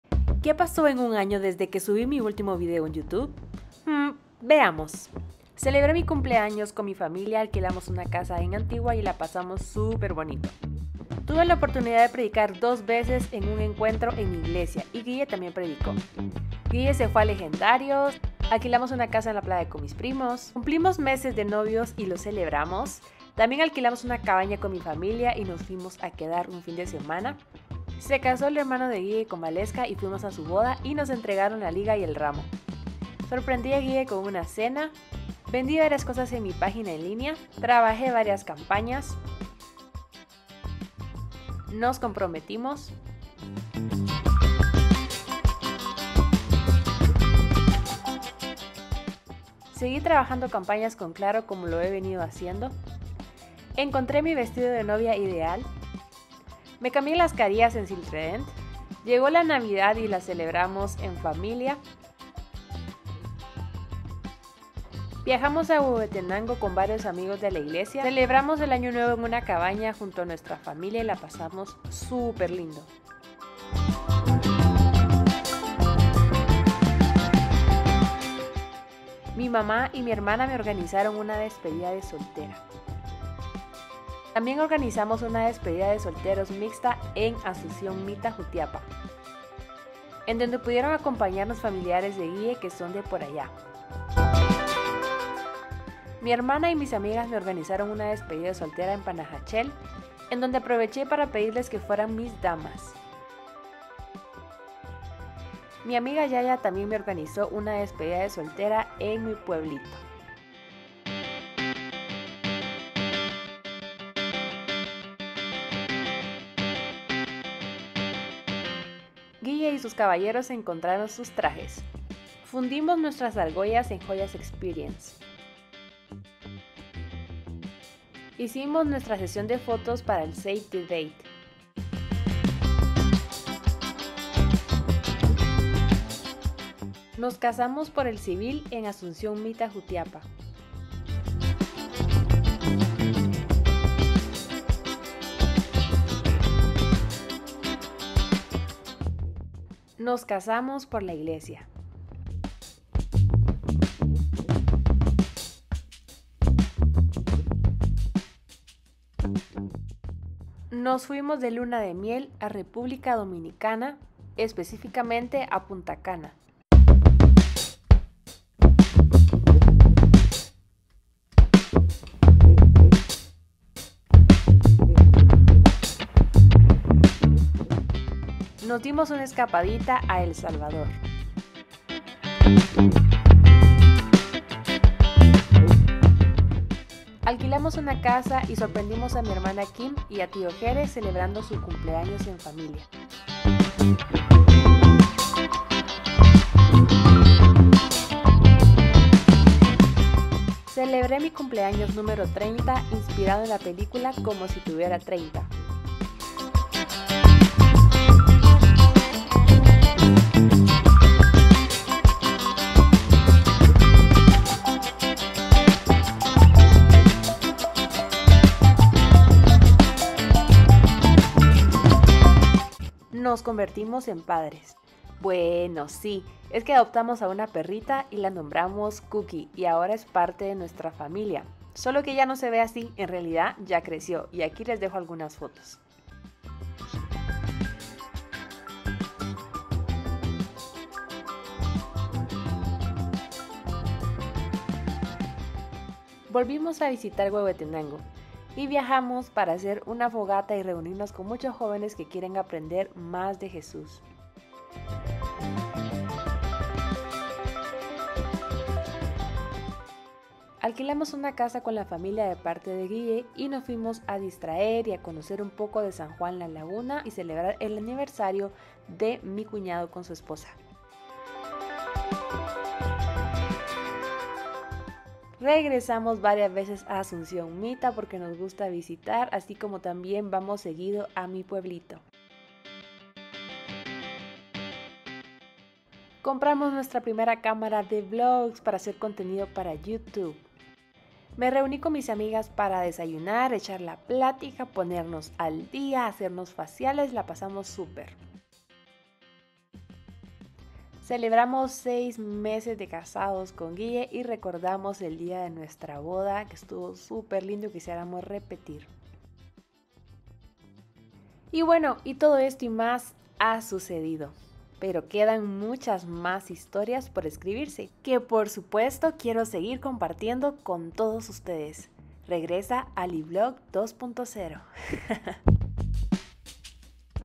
ESTOY DE VUELTA/ NOS CASAMOS/ nos convertimos en padres!
He vuelto :D estoy feliz de regresar a este proyecto que he amado desde el 2014. Gracias por estar aquí, espero que podamos coleccionar muchísimos recuerdos juntos. Déjame tus comentarios